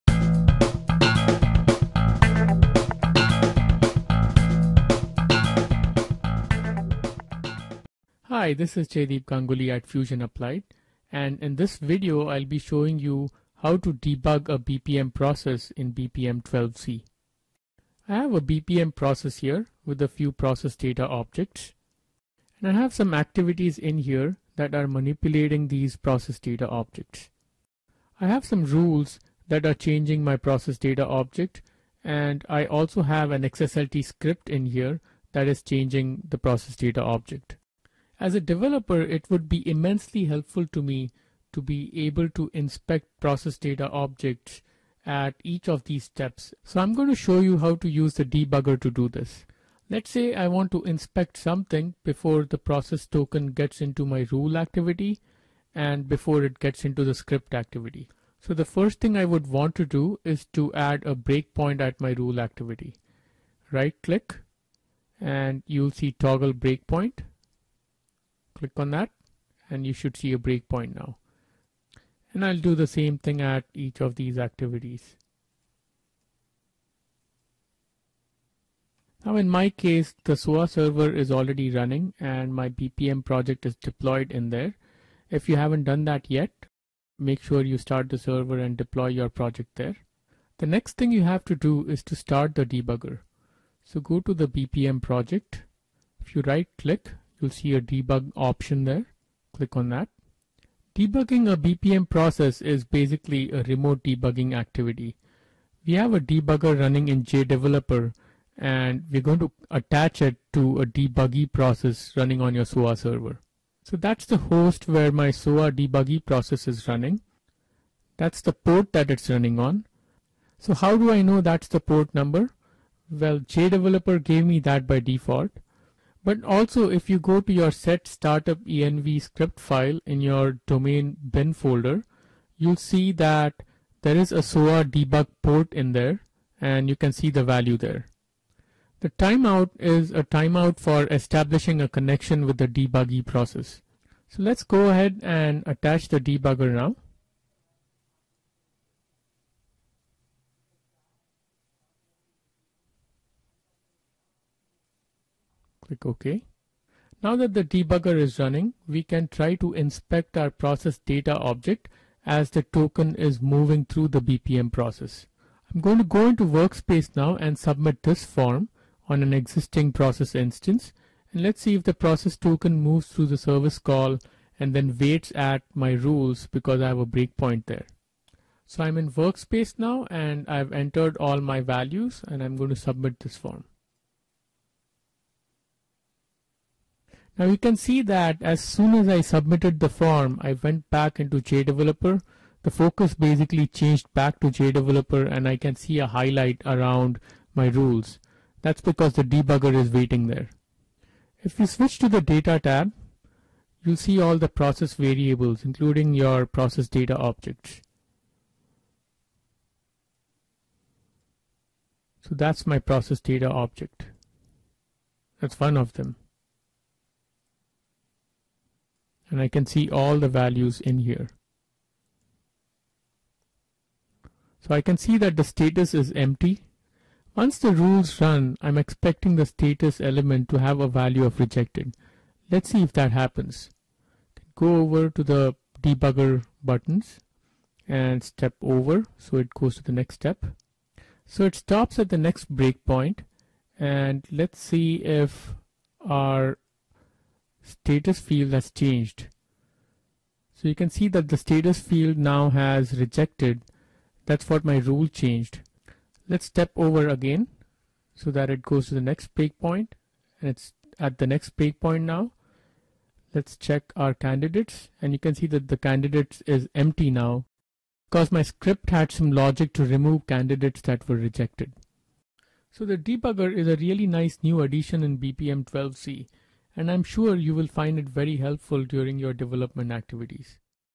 How to debug a BPM Process in BPM 12c
This video is an excerpt from one of our BPM Training courses that shows you how to use the new BPM process debugger in JDeveloper 12c

You can also sign up for Free Lessons and Training Discounts